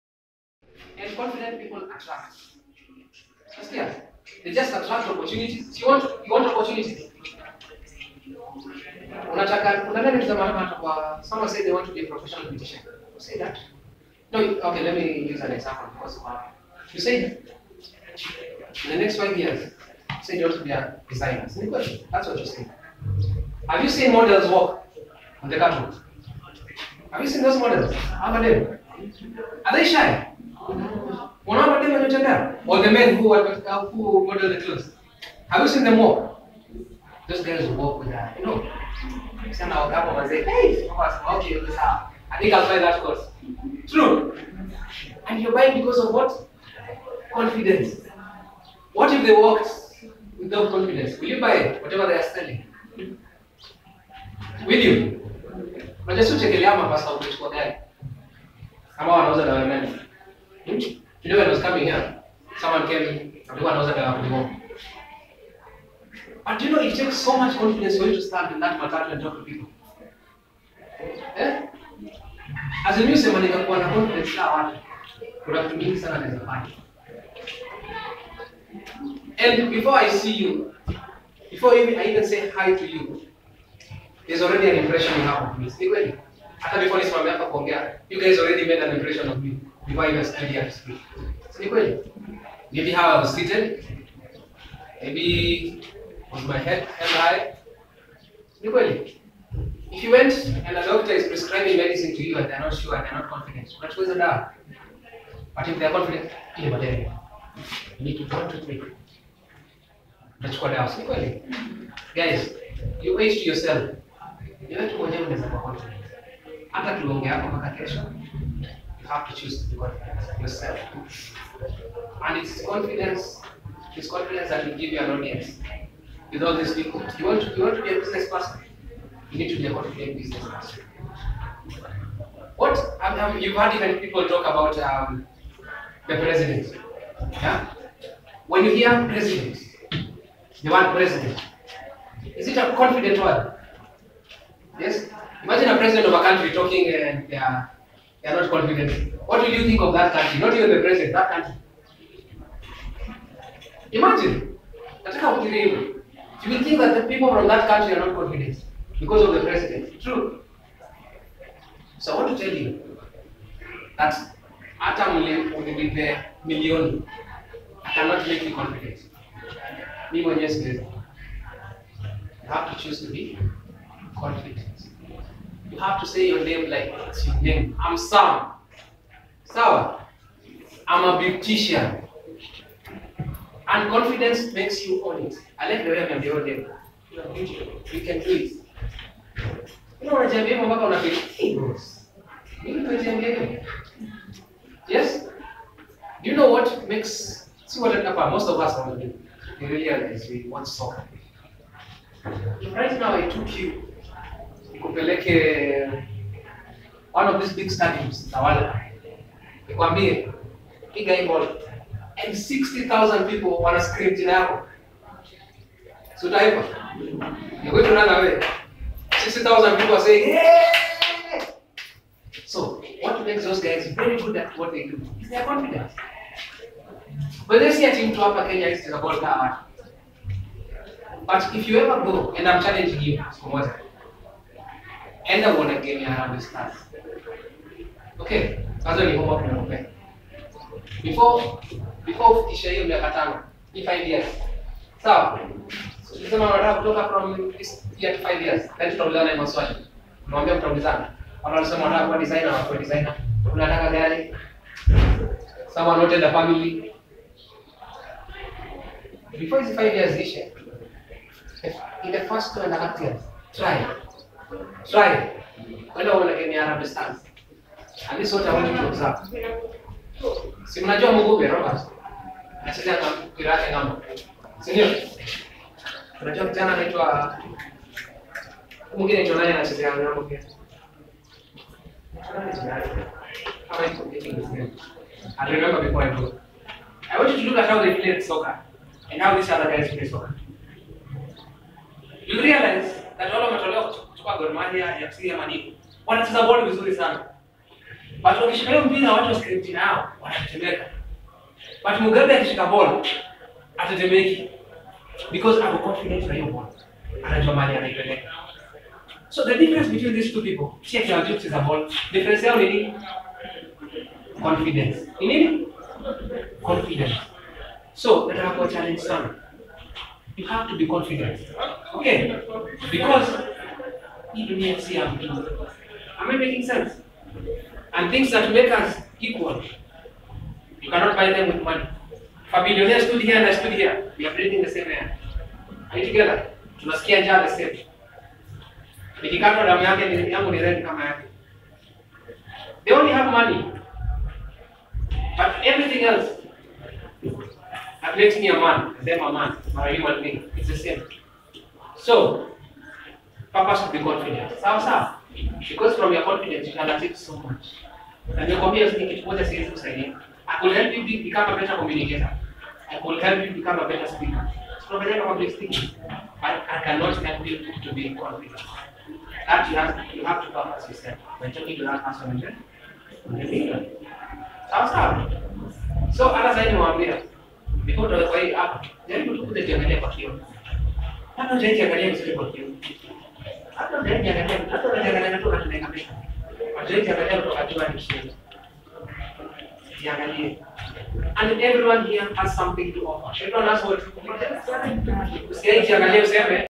Why self-confidence is critical for one to succeed in life
Team-building coordinator, Sam motivates the girls during a talk on why it is critical for one to have self confidence in order to succeed in life.